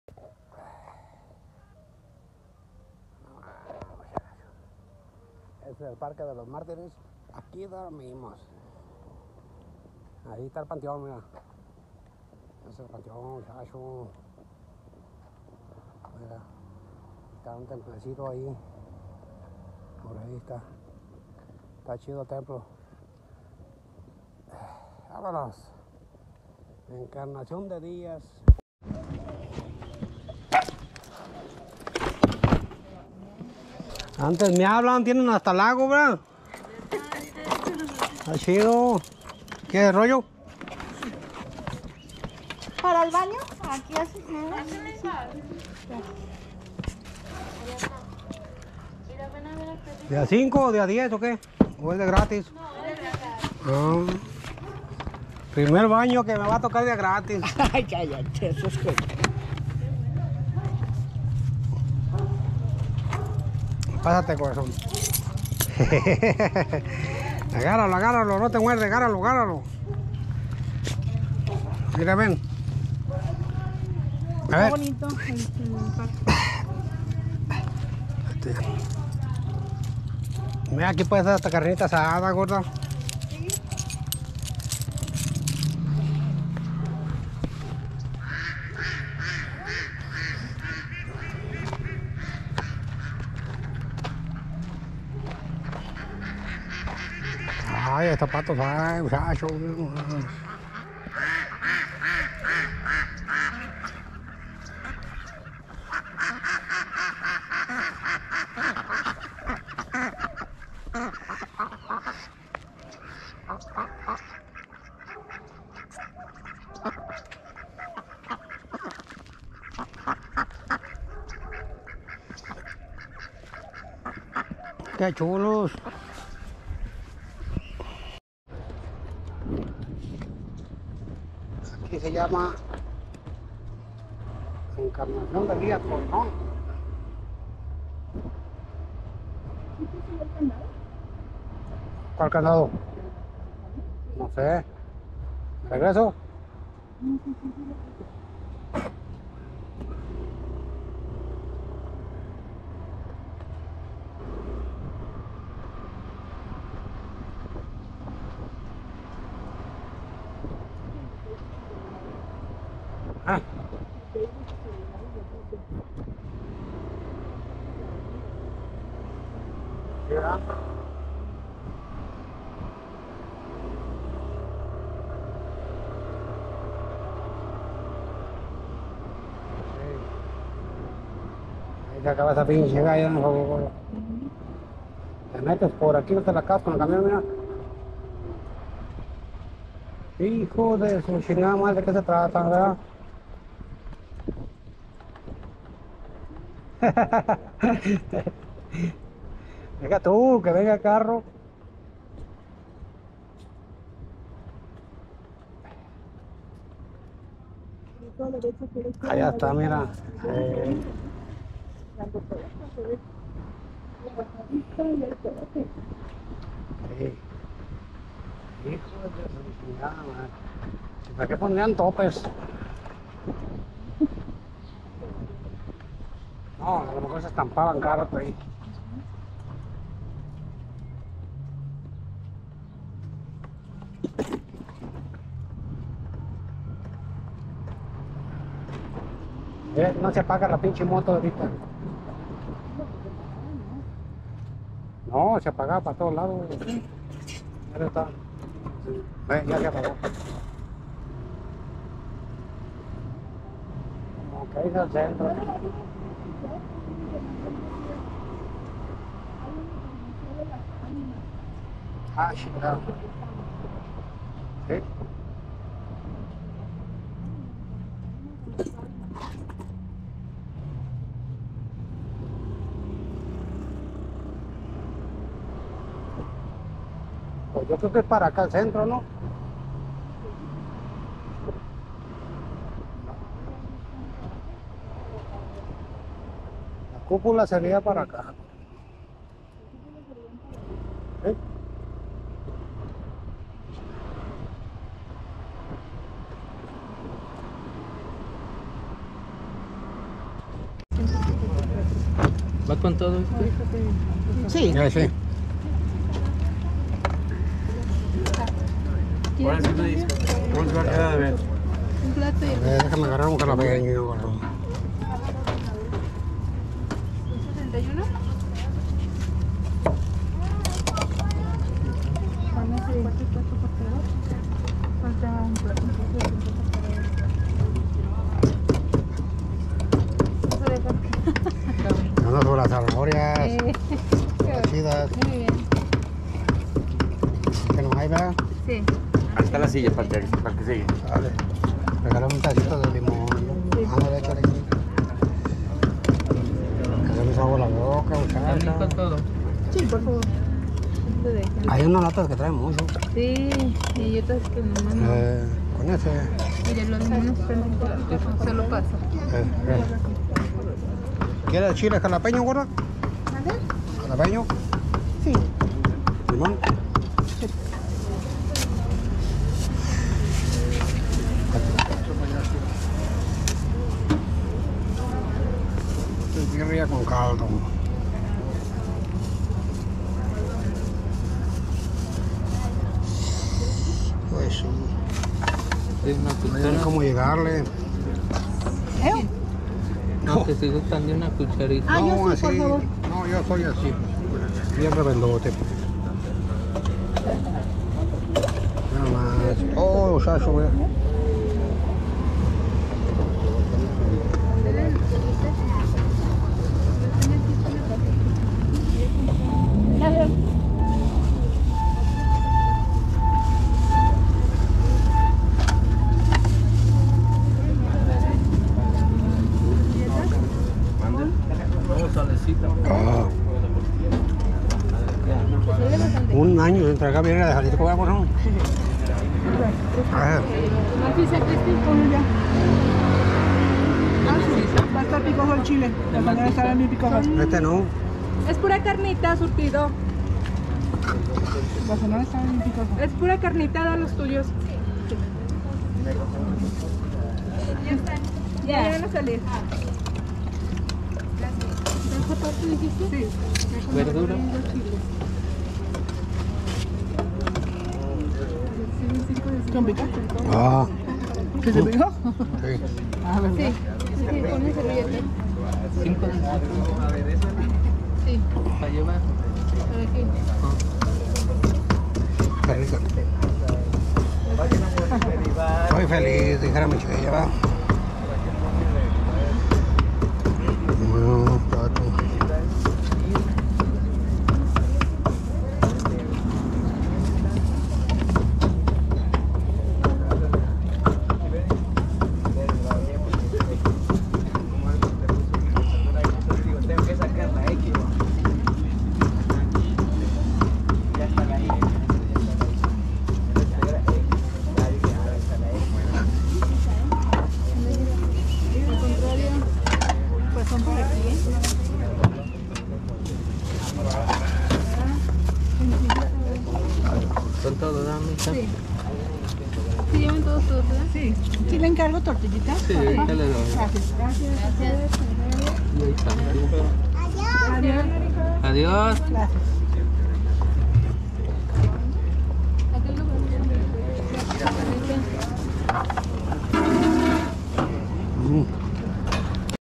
LA CHONA , JALISCO
Dormimos afuera del panteón